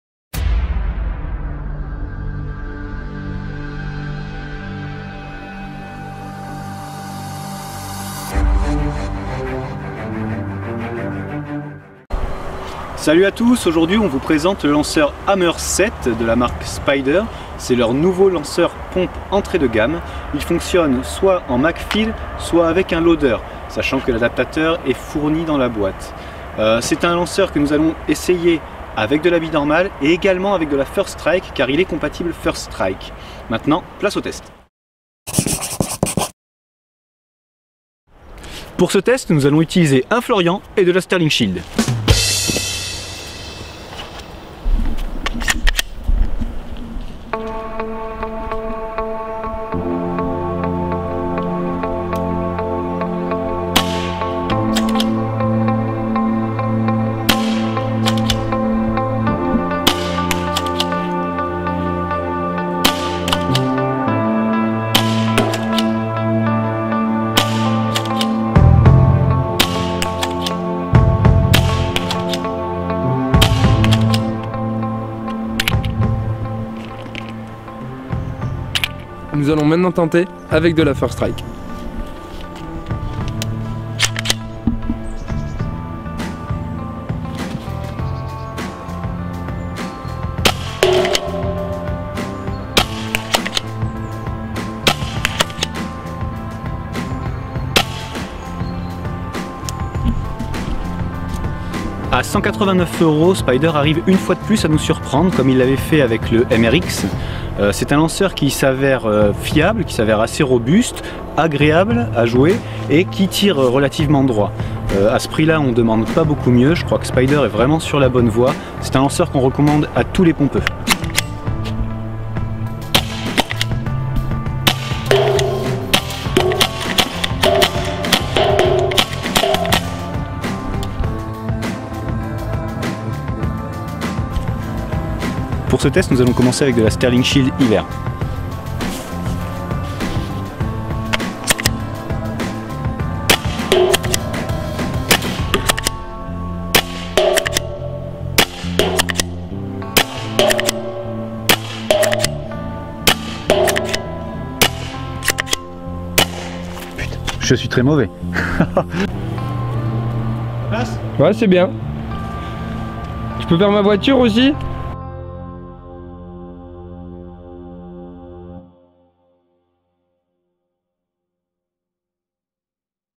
Paintballtv.fr / Test Spyder Hammer 7
Prise en main du Spyder Hammer 7, le nouveau lanceur pompe de chez KINGMAN.
Très surprenant pour de l'entrée de gamme, il s'avère précis et agréable à jouer.
Que du bon !